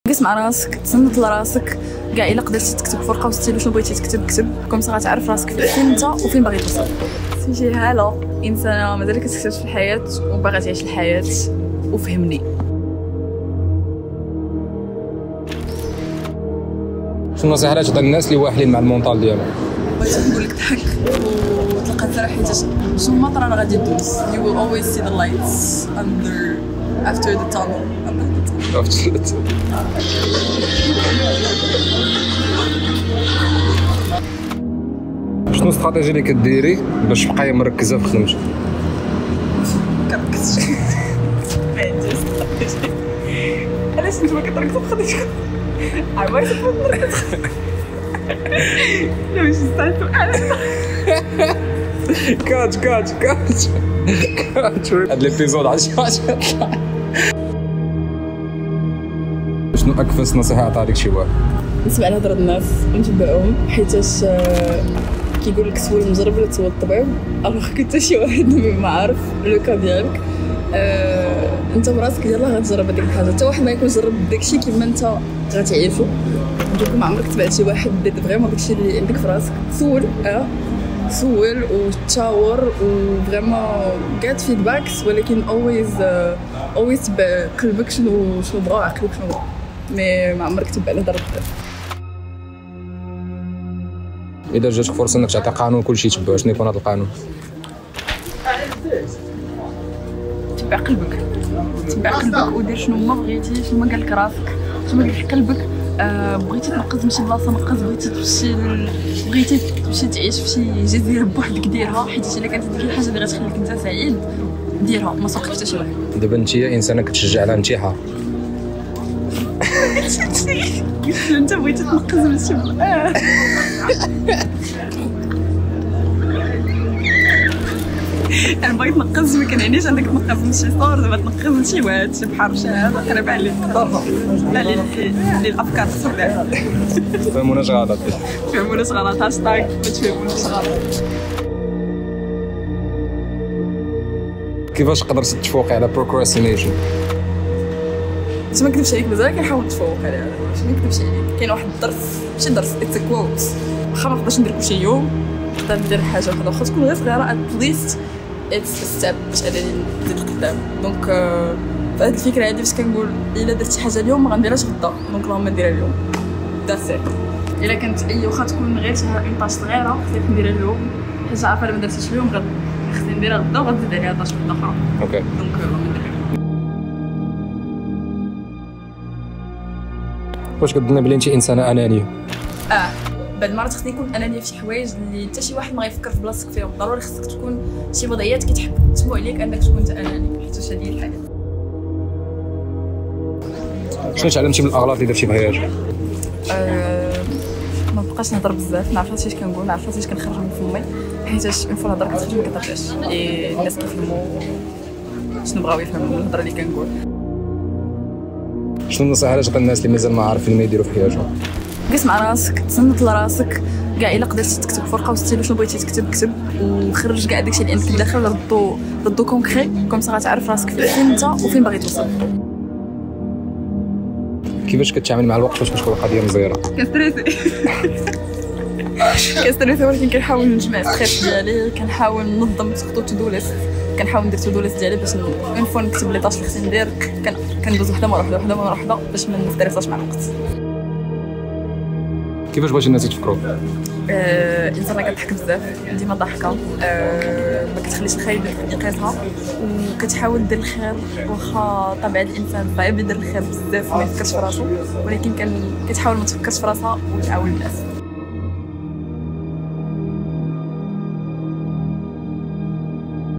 FHEMNI - The soulful Hala - طلقها تسرح - EP45
FHEMNI is an authentic, genuine daily ramadan series inviting mutual understanding, free from judgment or critique.

Had the privilege to discover the wisdom behind the pure soul, leading us to her way of thinking through his journey of life.

Directed, filmed and edited by ifanfilm

ifanfilm ig:  @ifanfilm
Hala instagram : @hala_aouad
Production : @ifanguer